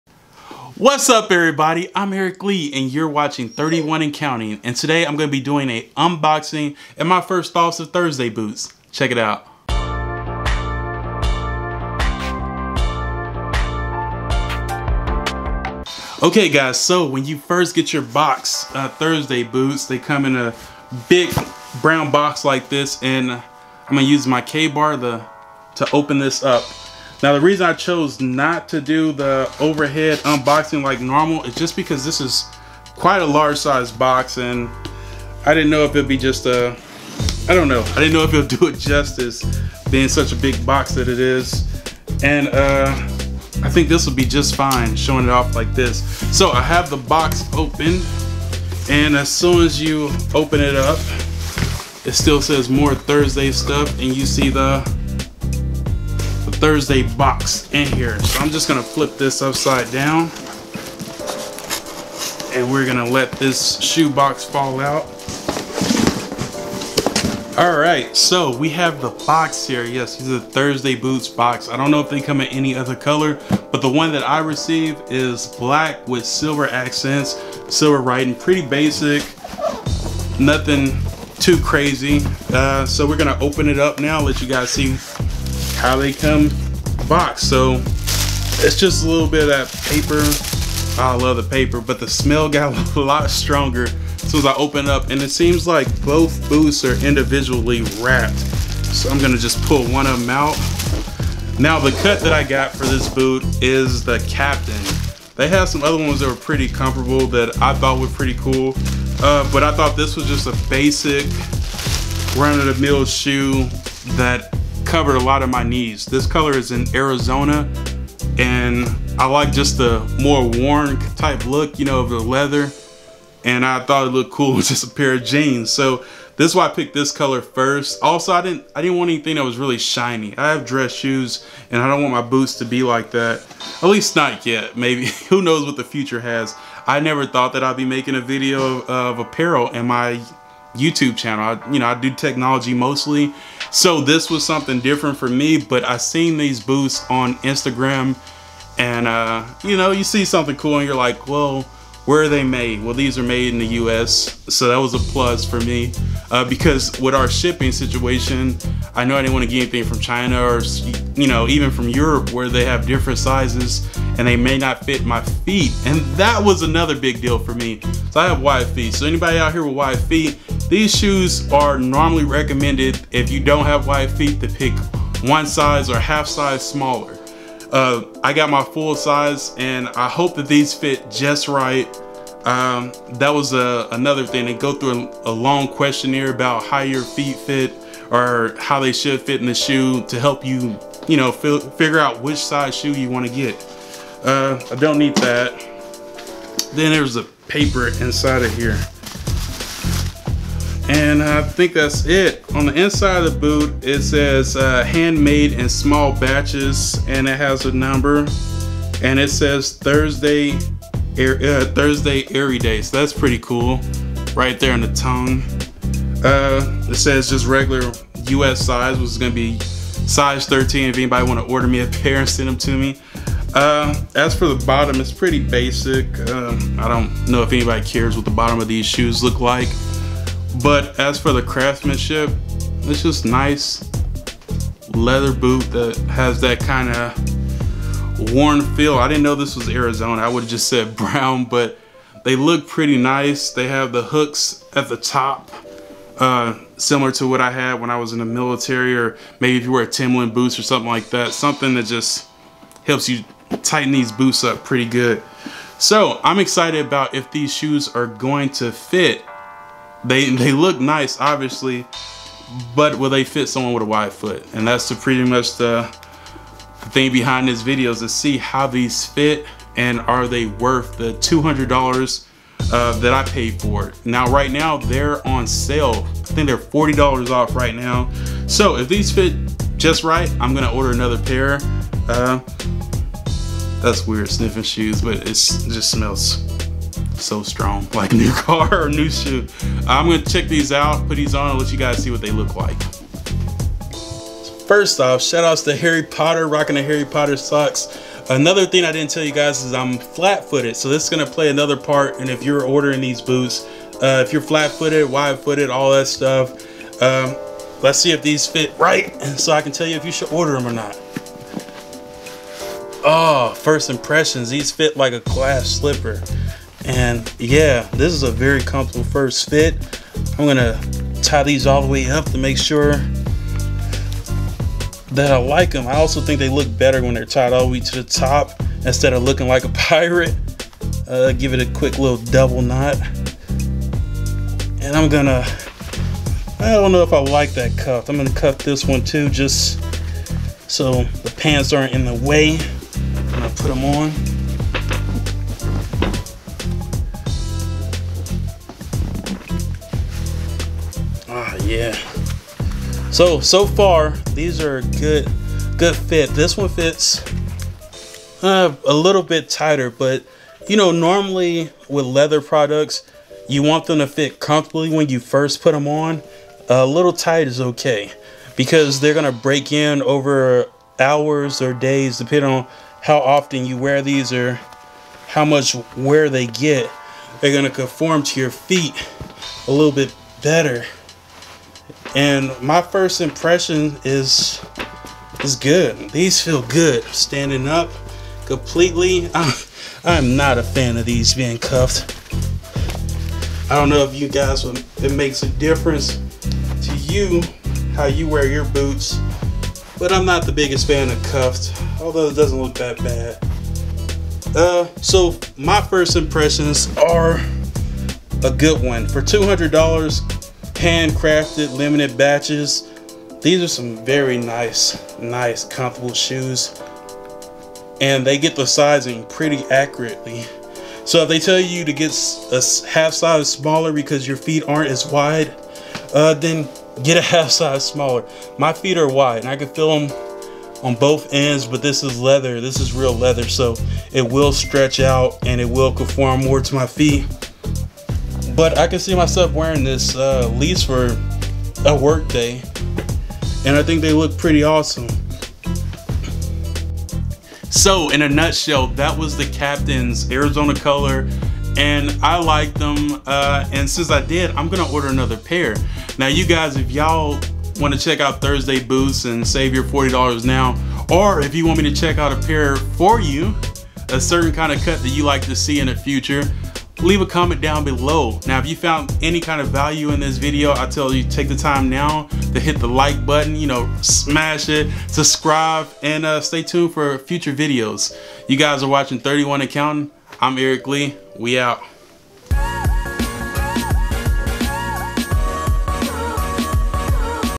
Thursday Boots review || Are they worth the price?
This is an unboxing and first thoughts of Thursday Boots.

My Gear:
📷Main Camera
📷Backup Camera

BUY DR. SQUATCH